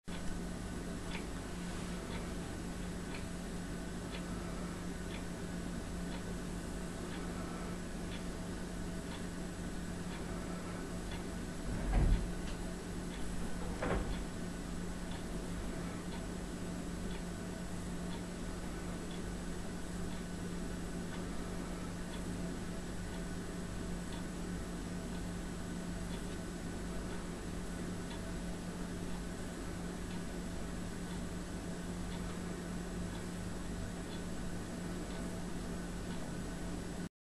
Multi girl Lapsit..
My friend had a go at seeing how many girls (full weight) could sit on his lap?